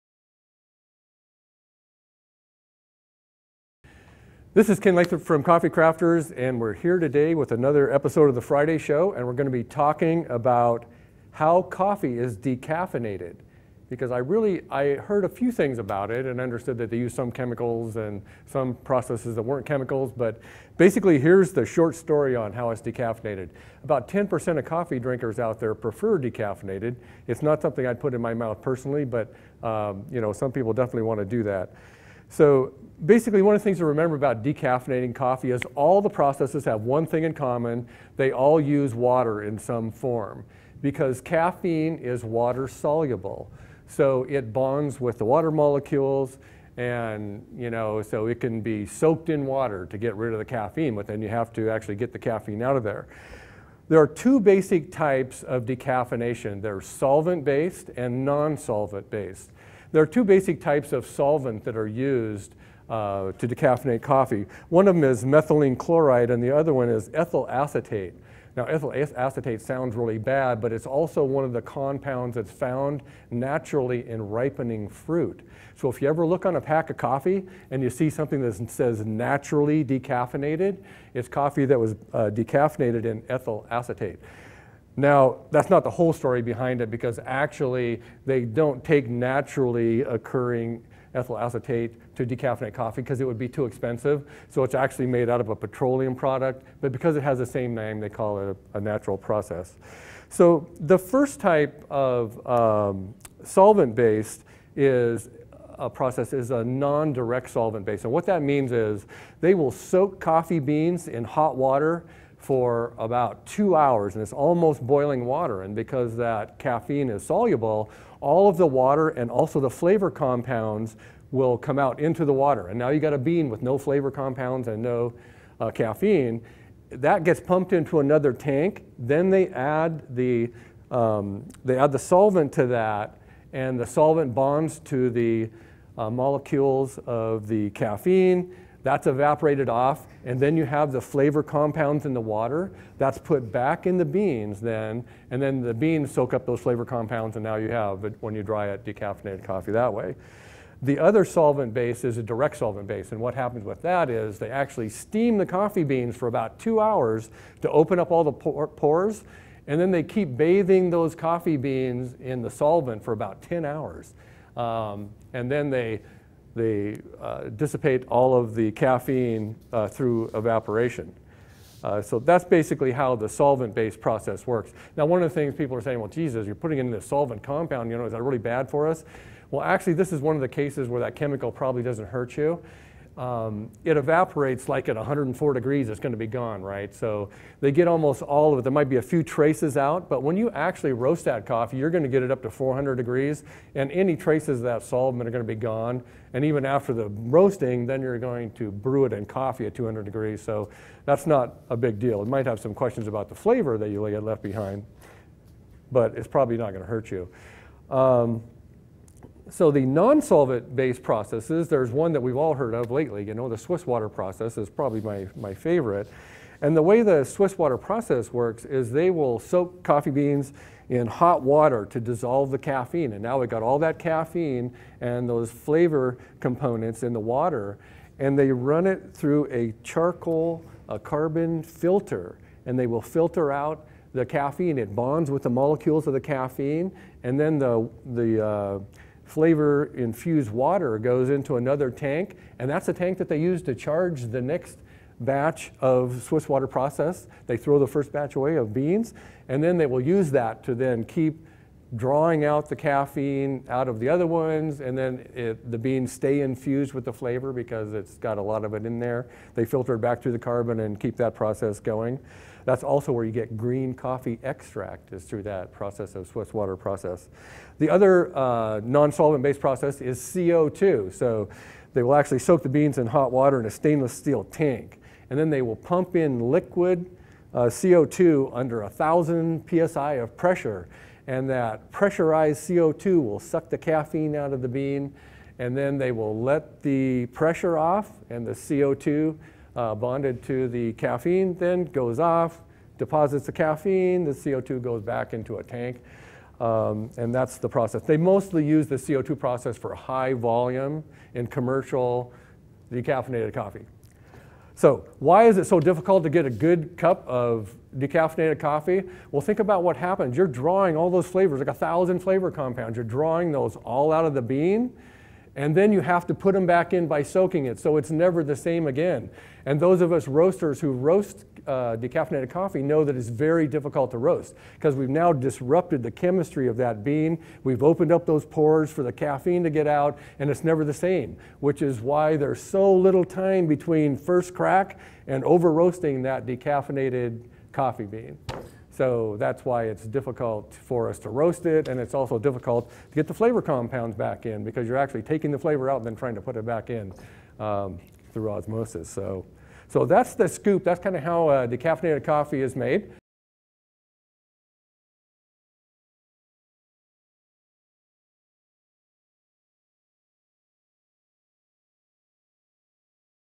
How Coffee is decaffeinated
Coffee can be decaffeinated in several ways. In this video, Ken from Coffee Crafters explains different processes and which one is his favorite decaf.

If advertising as 'all natural' is important to your market, you may opt for the Swiss Water Process decaf. If your market is more interested in better flavor, you may opt more for the MC processed decaf.